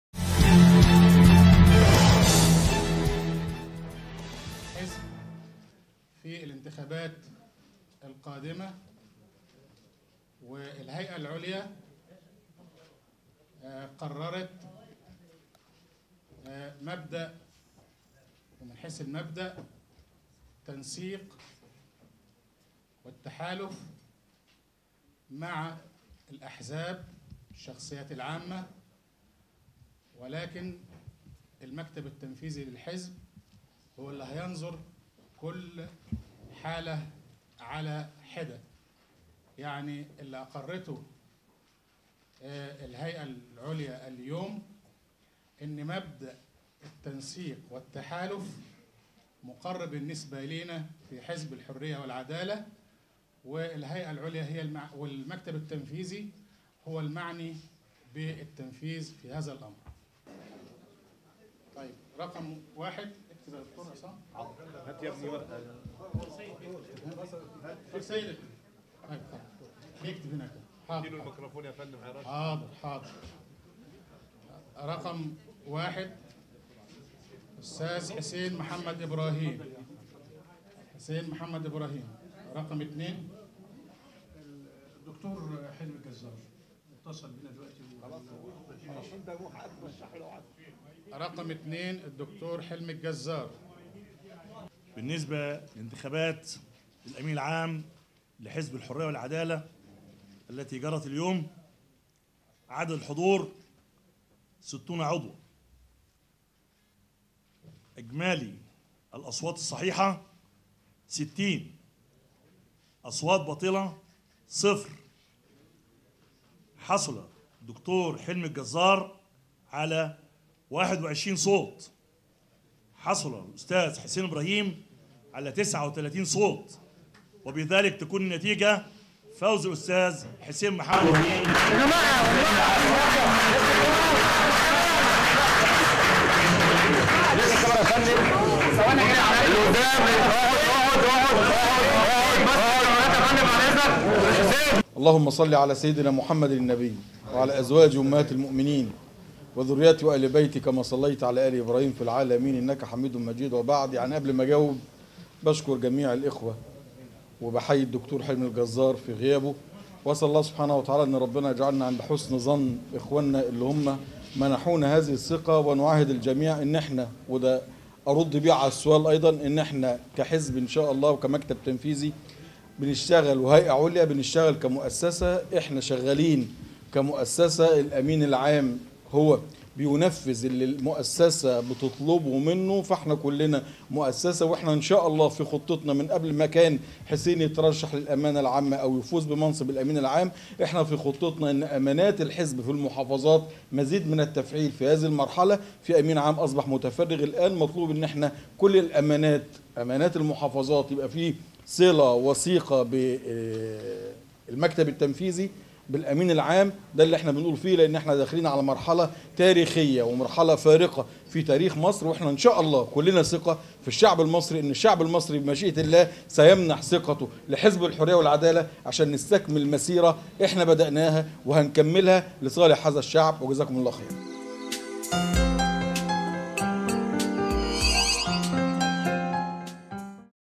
ملخص انتخابات الامانة العامة لحزب الحرية والعدالة
رصد | ملخص انتخابات الامانة العامة لحزب الحرية والعدالة

متابعة : حسام زيدان
تصوير : عمر خضر
مونتاج : نور جلال